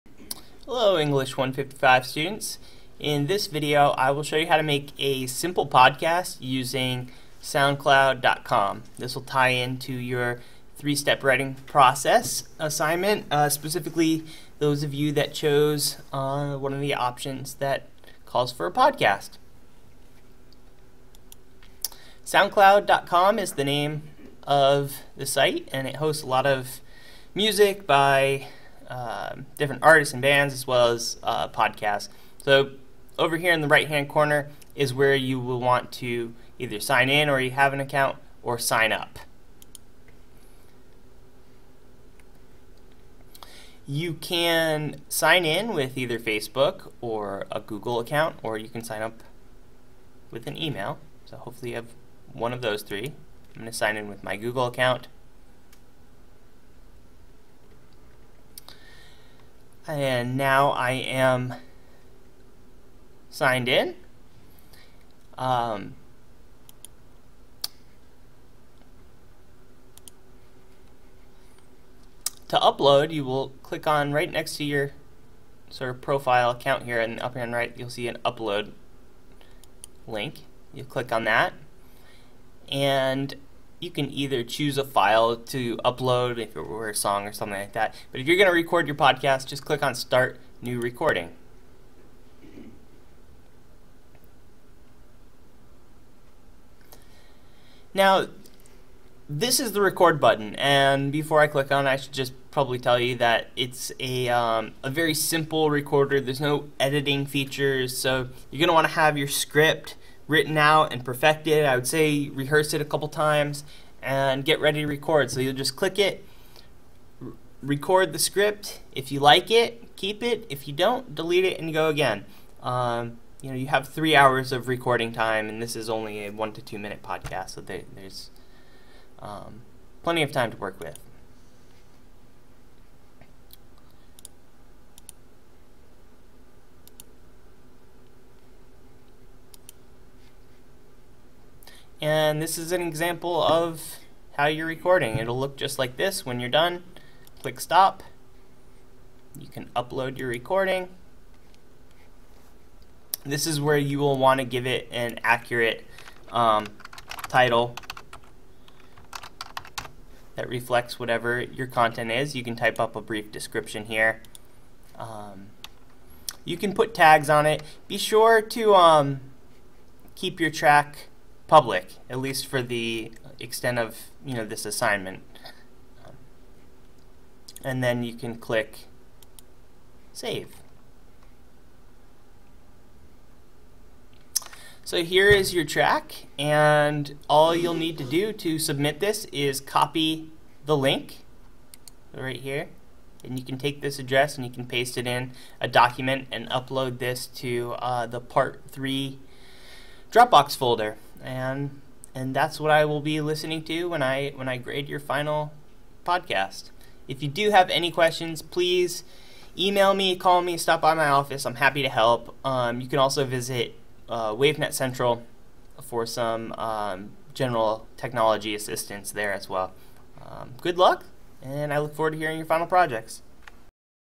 How To: Create a Simple Podcast Using SoundCloud
ENG 155 Tutorial: how to use to make an audio recording for your 3-Step Writing Assignment.